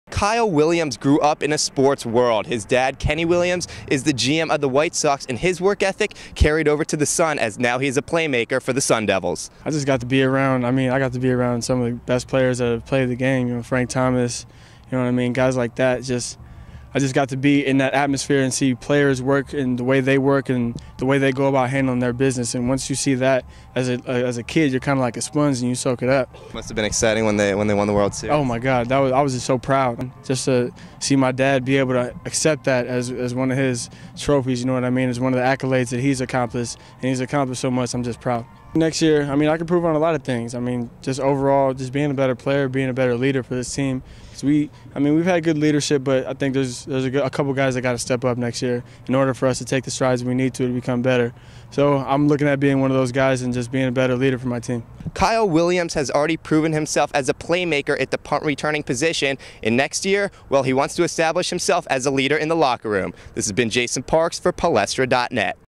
Working For Respect At Arizona State
Arizona State wide receiver Kyle Williams is the son of the owner of the Chicago White Sox, but is playing for respect. Palestra.net's Jason Parks talked to him about growing up in the sports world and what he wants to do at Arizona State.